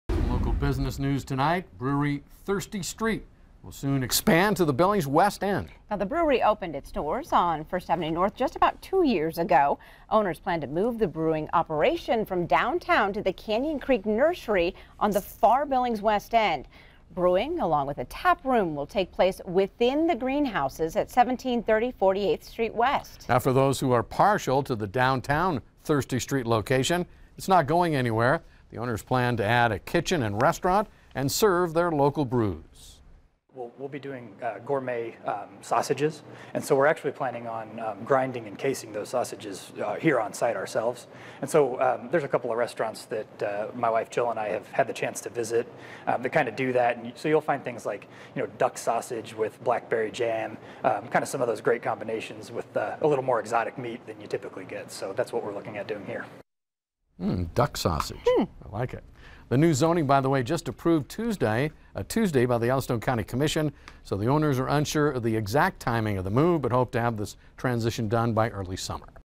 Thirsty Street Brewing Co. to expand to West End, add restaurant
Thirsty Street and Get Juiced